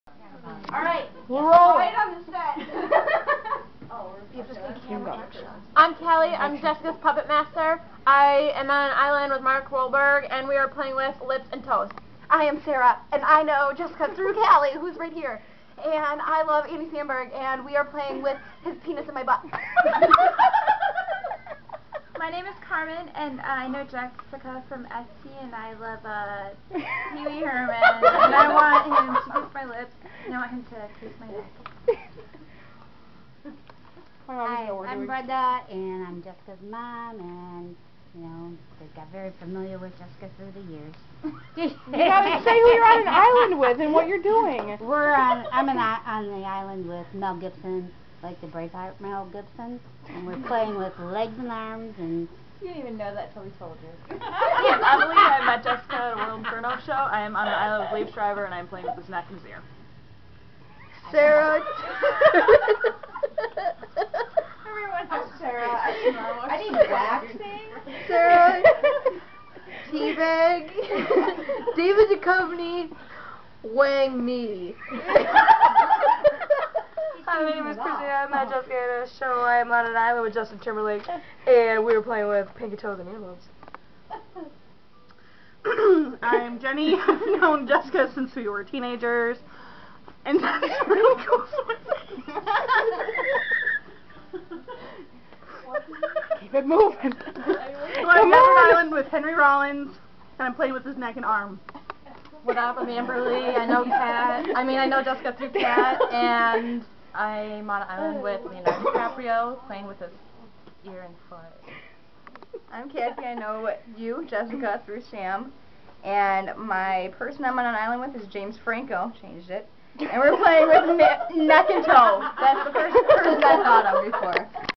Pure Romance Game, recap!
In this video I force everyone to repeat their answers to a game in which people had to list their name, how they know me, a celebrity that they're stuck on an island with and the body parts they're playing with